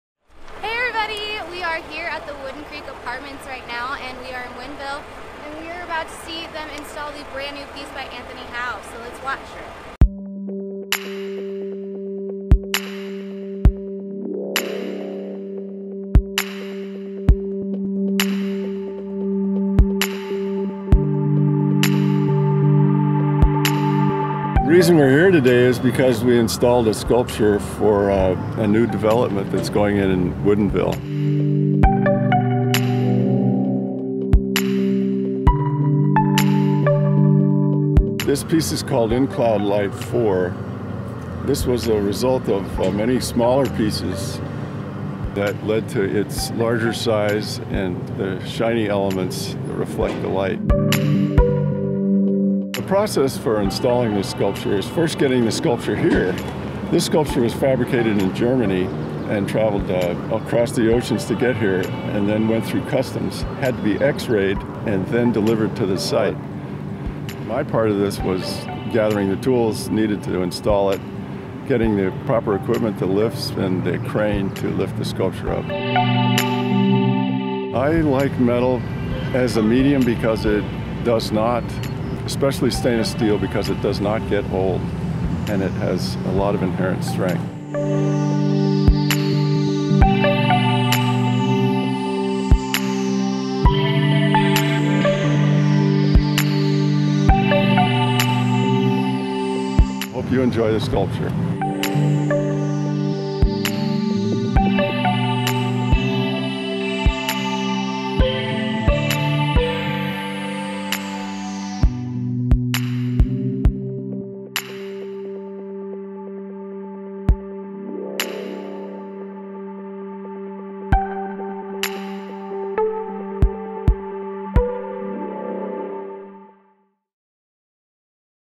Installation of In Cloud Light IV, and interview with Anthony Howe
Watch the quick installation of In Cloud Light IV at our community, Woodin Creek Village, in Woodinville, WA. Plus, we have an exclusive interview with the artist, Anthony Howe, on this beautiful sculpture